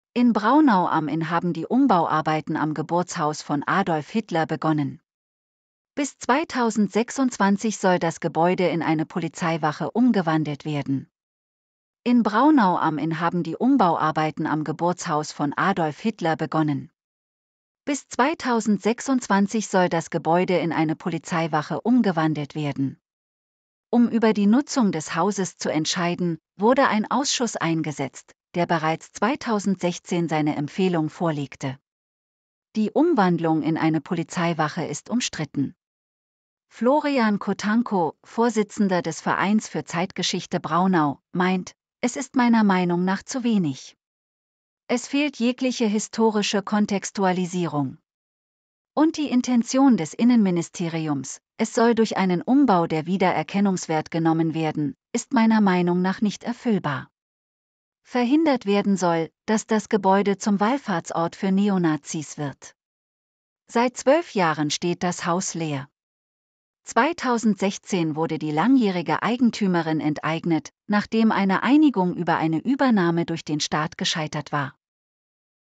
Umbau des Hitler-Geburtshauses in Braunau: Die Bauarbeiten haben begonnen
In Braunau am Inn haben die Umbauarbeiten am Geburtshaus von Adolf Hitler begonnen.